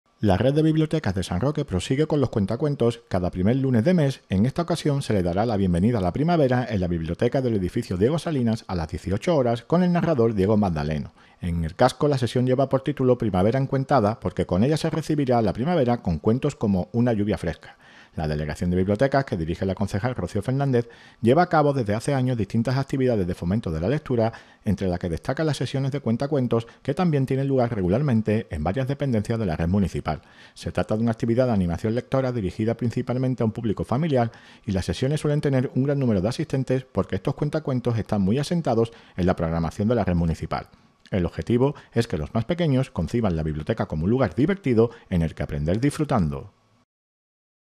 NOTICIA PREVIA CUENTOS ENCUENTADOS 3 MAYO 2024
La Red de Bibliotecas de San Roque prosigue con los cuentacuentos, cada primer lunes de mes, en esta ocasión se le dará la bienvenida a la Primavera en la Biblioteca del Edificio Diego Salinas, a las 18 horas, con el narrador Diego Magdaleno.